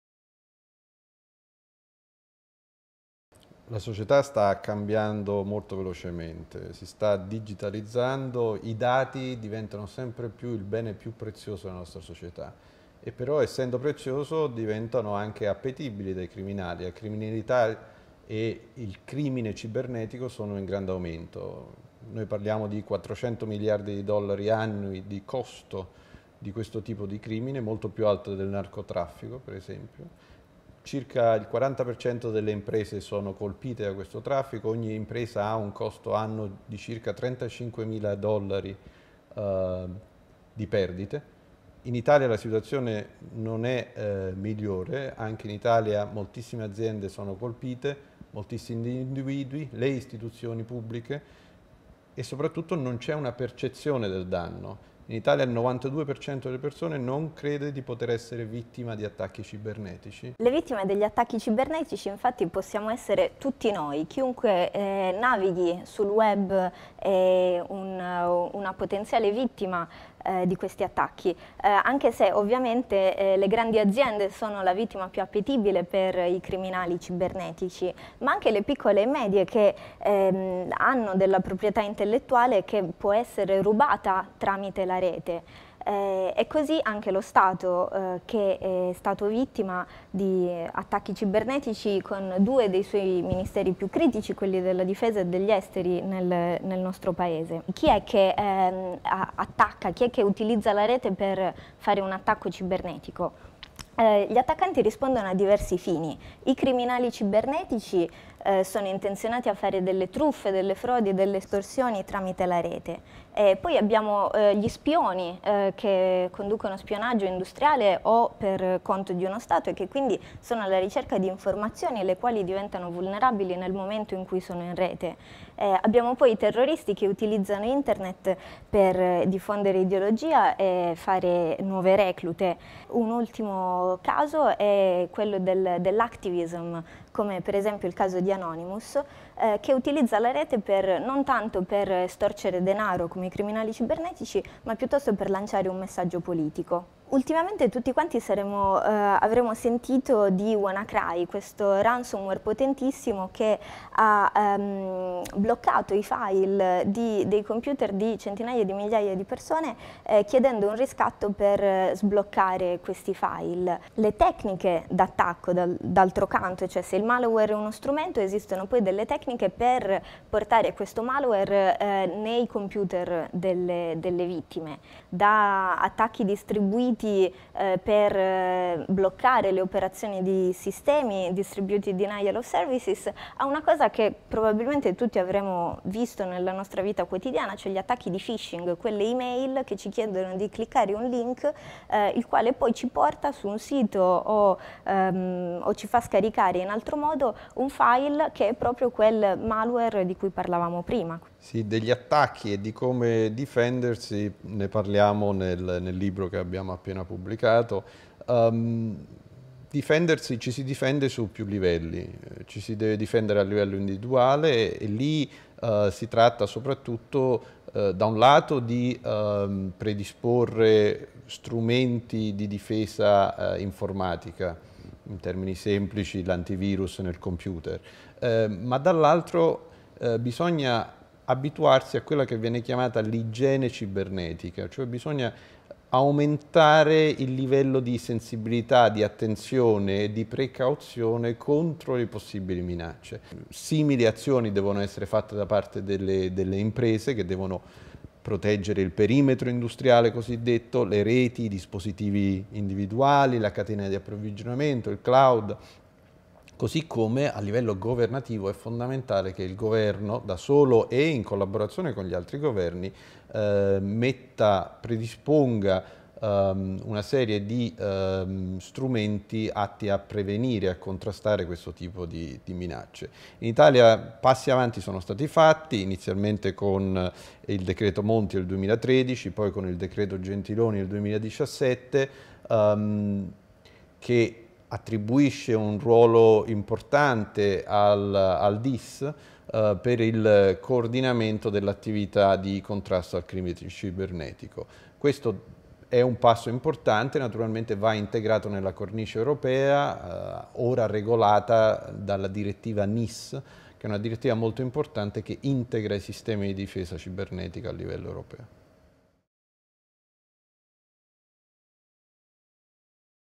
LUISS Open: Cyber security e le minacce sul web
I docenti LUISS Raffaele Marchetti e Roberta Mulas presentano il libro Cyber security: Hacker, terroristi, spie e le nuove minacce del web per il research magazine LUISS Open. Pubblicato da LUISS University Press, il volume racconta la nascita e l'evoluzione dello spazio cibernetico e della sicurezza informatica, passando in rassegna le varie tipologie di minaccia a cui siamo esposti e delineando le possibili forme di difesa a disposizione di istituzioni, aziende e cittadini.